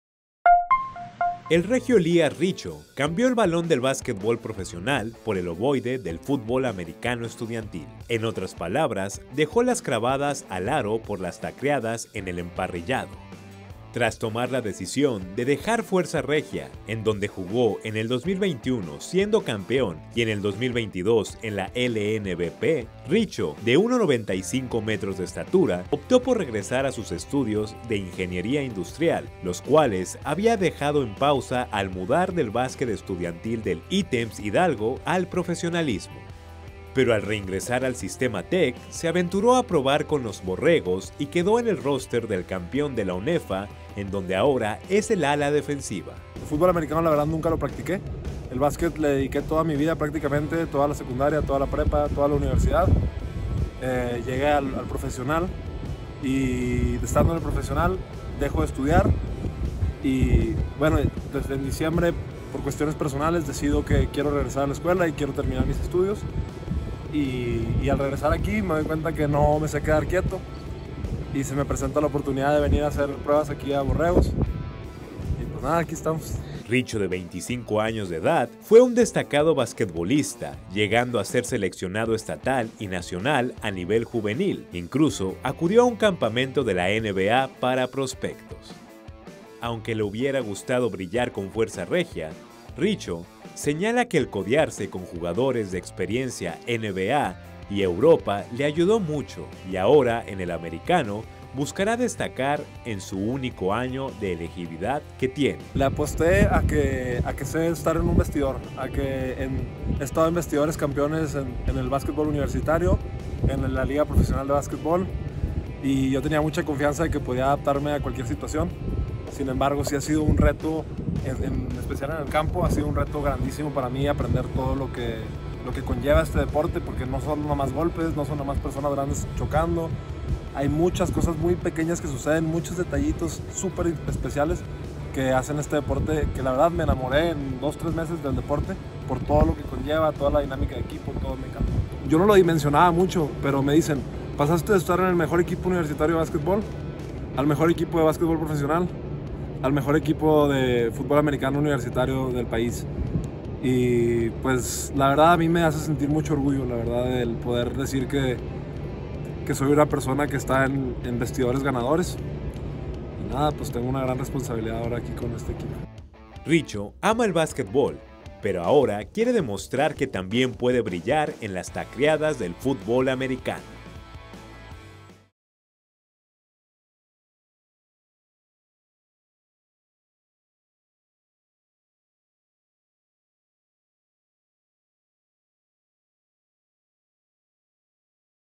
Elías Richo cambia Fuerza Regia por Borregos
Elías Richo, ex jugador de Fuerza Regia, busca demostrar su talento en el futbol americano con los Borregos.